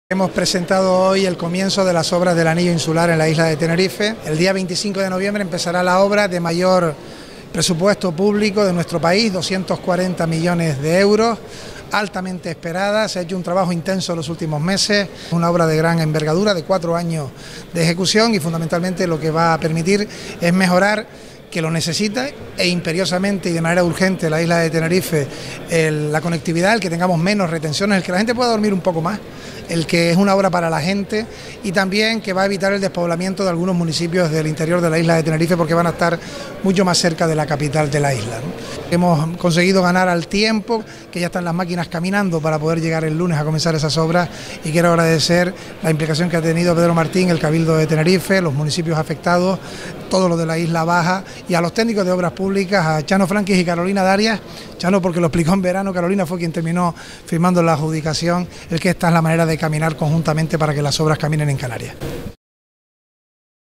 Anillo insular de Tenerife
Las obras del anillo insular de Tenerife comenzarán el próximo 25 de noviembre. El presidente Ángel Víctor Torres presenta los detalles del proyecto, con 48 meses de duración y que conectará la TF-1 con la TF-5 por el noroeste (El Tanque y Santiago del Teide).

Fecha: 20 de noviembre de 2019
Lugar: Santa Cruz de Tenerife
Créditos: Presidencia del Gobierno